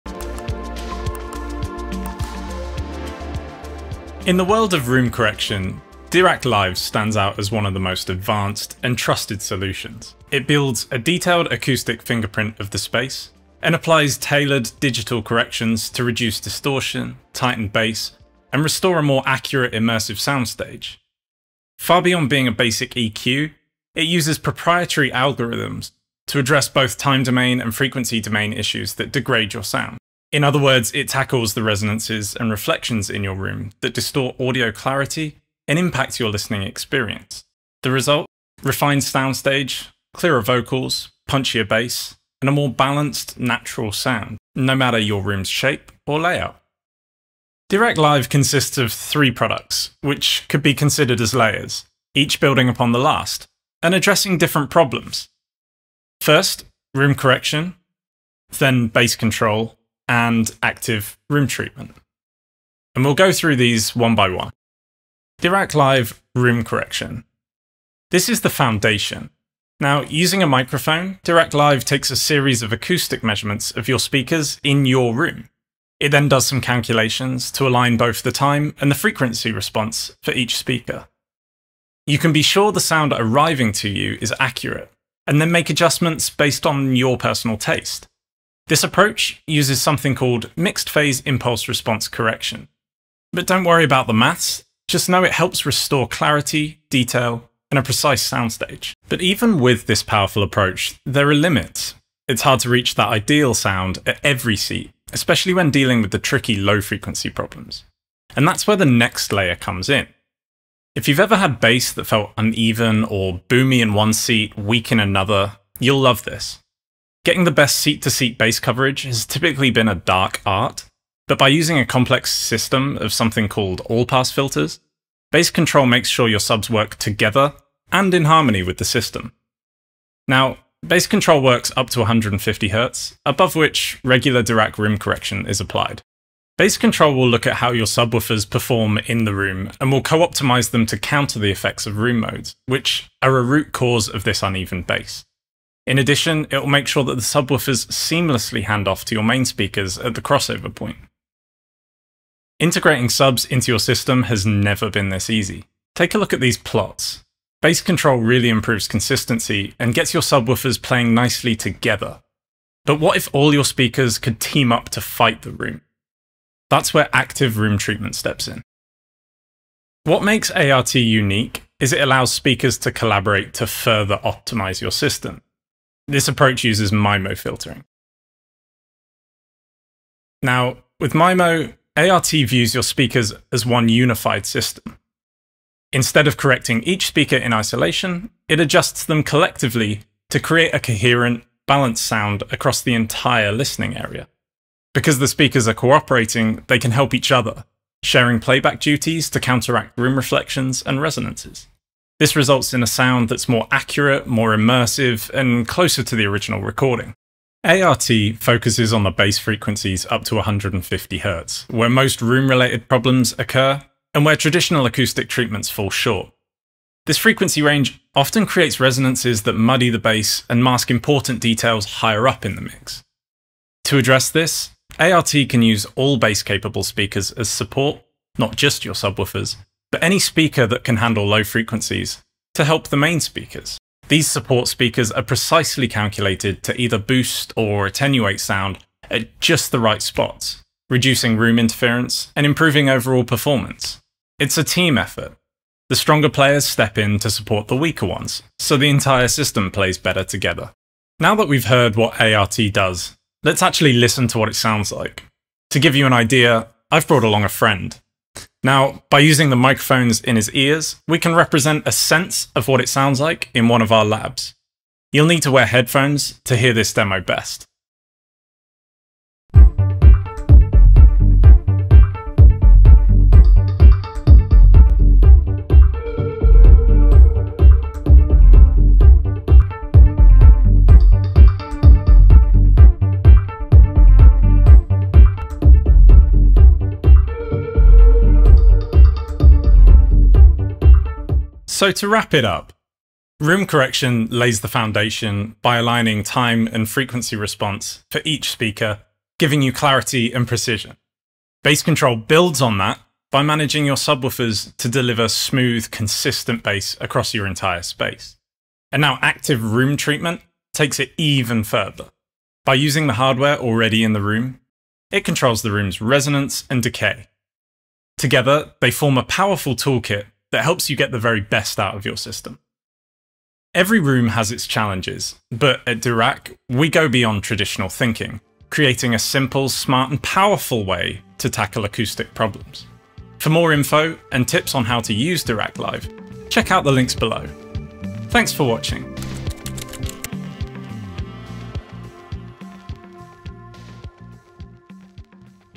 Dirac Live Explained
What is Dirac Live?

In this video, Joss Walker dives into what makes Dirac Live one of the most advanced room correction solutions available today. He explains how it builds a precise acoustic fingerprint of your room and applies intelligent digital corrections to reduce distortion, tighten bass, and restore a more immersive soundstage.

Joss walks through the three layers of the Dirac Live suite:

Room Correction
Bass Control
Active Room Treatment

Far beyond a simple EQ, Dirac Live uses proprietary algorithms to tackle both time and frequency domain issues—addressing resonances, reflections, and other factors that distort clarity. The result? Clearer vocals, punchier bass, and a more natural, balanced sound, no matter your room layout.

Whether you're an audiophile or simply want better sound from your system, this is your starting point to understand how Dirac Live transforms your listening experience.